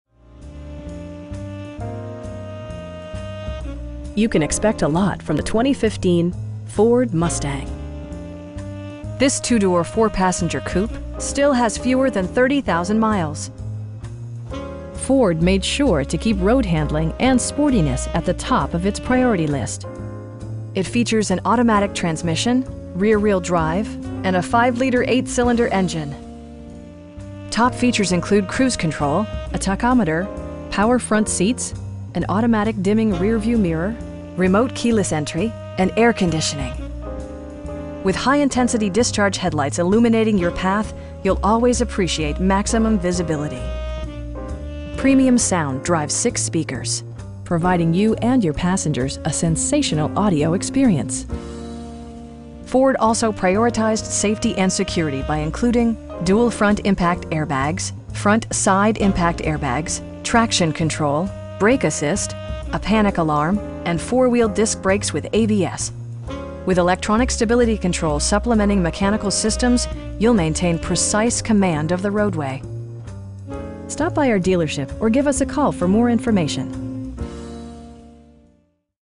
2015 Ford Mustang GT in Lubbock, TX 79407
You can expect a lot from the 2015 Ford Mustang. This 2 door, 4 passenger coupe still has fewer than 30,000 miles! Ford made sure to keep road-handling and sportiness at the top of it's priority list. It features an automatic transmission, rear-wheel drive, and 5 liter 8 cylinder engine. Top features include cruise control, a tachometer, power front seats, an automatic dimming rear-view mirror, front bucket seats, tilt and telescoping steering wheel, remote keyless entry, and air conditioning. With high intensity discharge headlights illuminating your path, you'll always appreciate maximum visibility. Premium sound drives 6 speakers, providing you and your passengers a sensational audio experience. Ford also prioritized safety and security by including: dual front impact airbags, front SIDE impact airbags, traction control, brake assist, a panic alarm, and 4 wheel disc brakes with ABS. With electronic stability control supplementing mechanical systems, you'll maintain precise command of the roadway. We pride ourselves in the quality that we offer on all of our vehicles. Stop by our dealership or give us a call for more information.